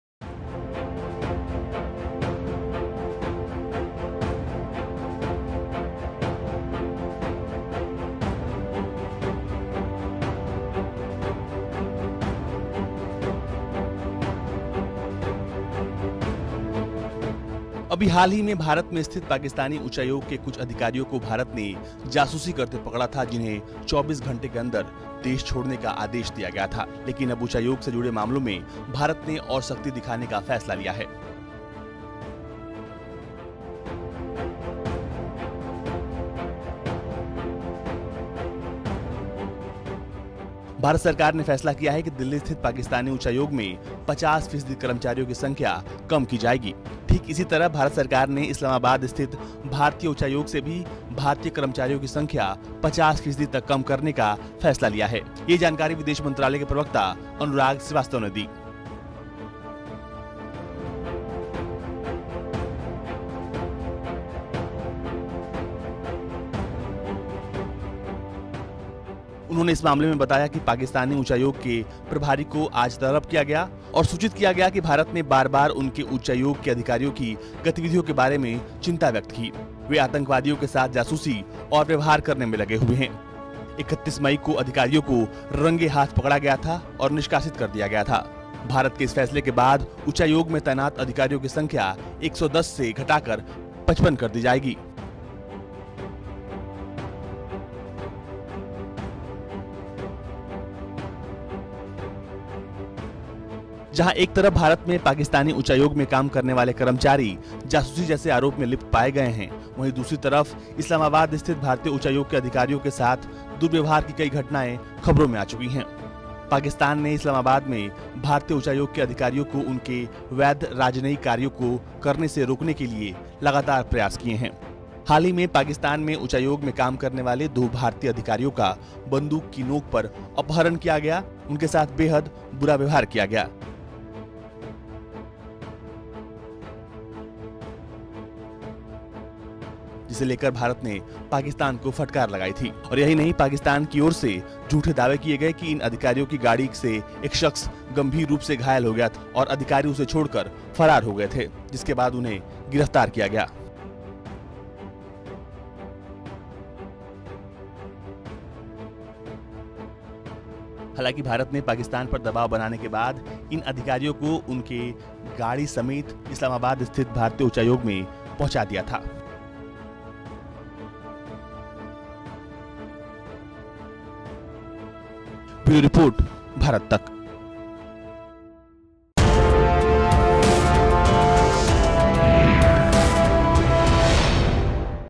पाकिस्तान को लेकर मोदी सरकार का एक और एक्शन
पाकिस्तानी उच्चायोग के कुछ अधिकारियों को भारत ने जासूसी करते पकड़ा था जिन्हें 24 घंटे के अंदर देश छोड़ने का आदेश दिया गया था। लेकिन अब उच्चायोग से जुड़े मामलों में भारत ने और सख्ती दिखाने का फैसला लिया है। भारत सरकार ने फैसला किया है कि दिल्ली स्थित पाकिस्तानी उच्चायोग में 50 फीसदी कर्मचारियों की संख्या कम की जाए।